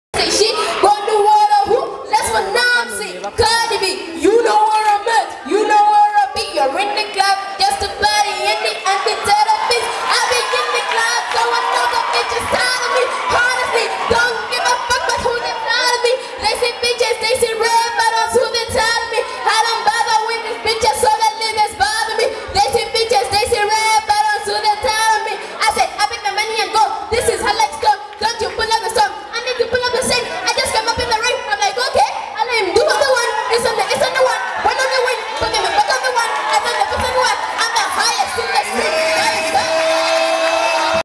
Uganda's Cardi B
Veronin is a naturally gifted rapper Cardi B free style
Enjoy her brain art